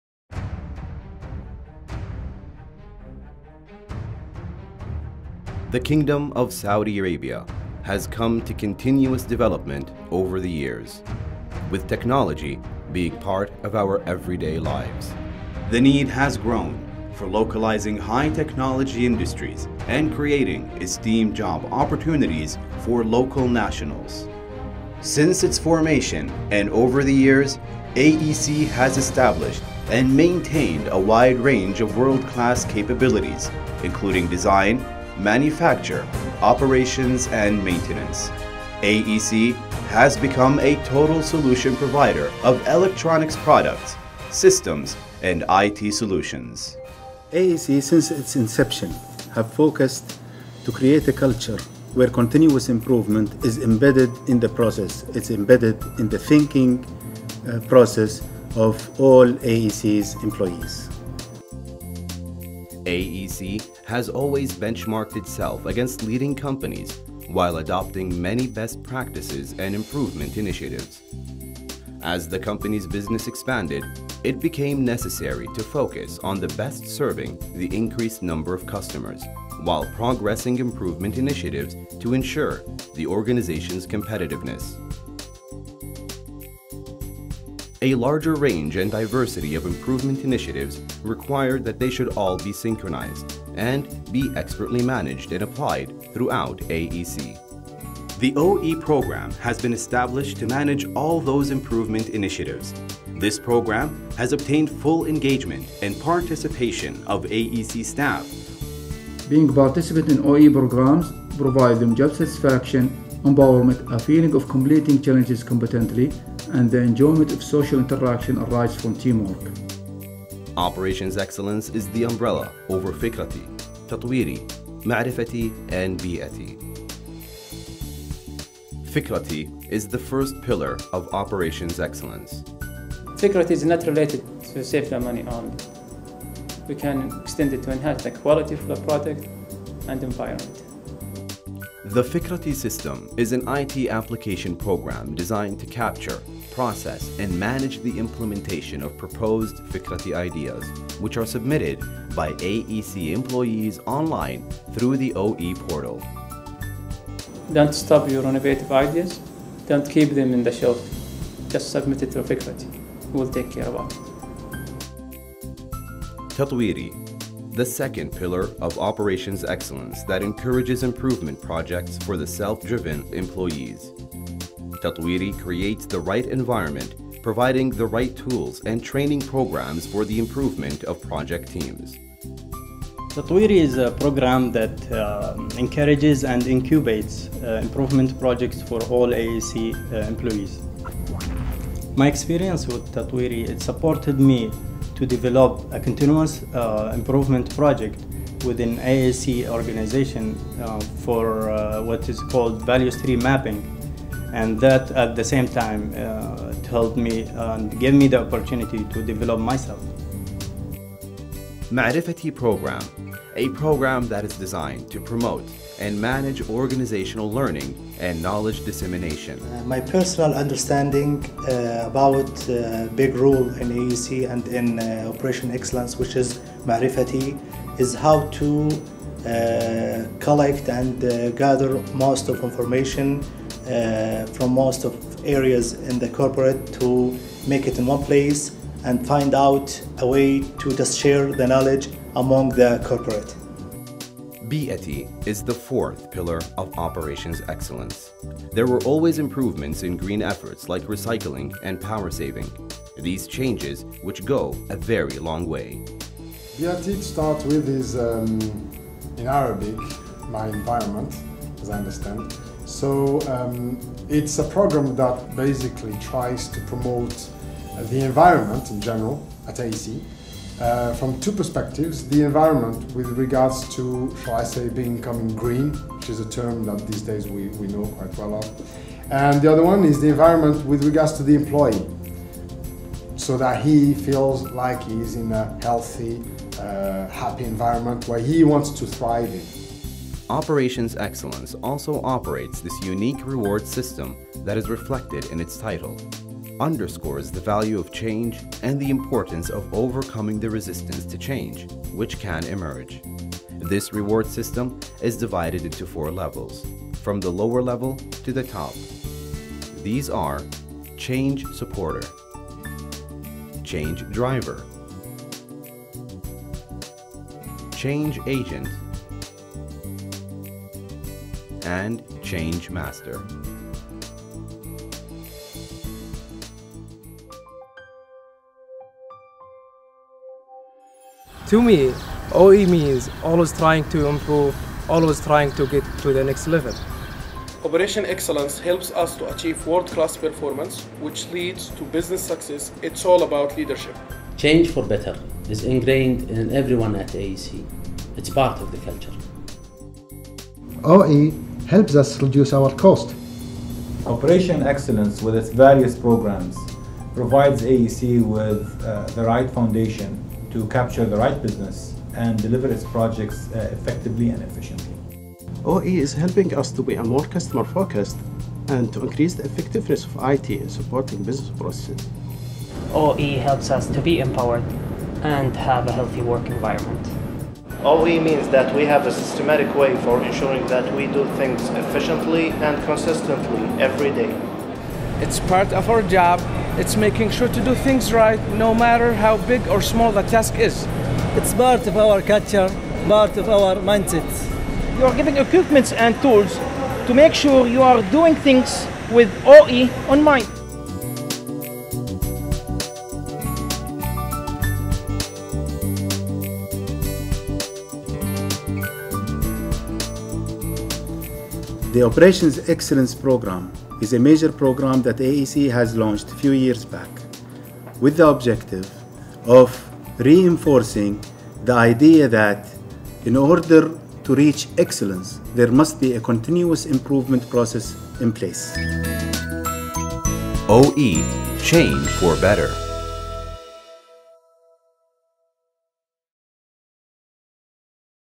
Operations Excellence in Advanced Electronics Company
Operations Excellence in AEC. Major achievements, programs and projects. Interviews with Advanced Electronics Company  employees and senior management.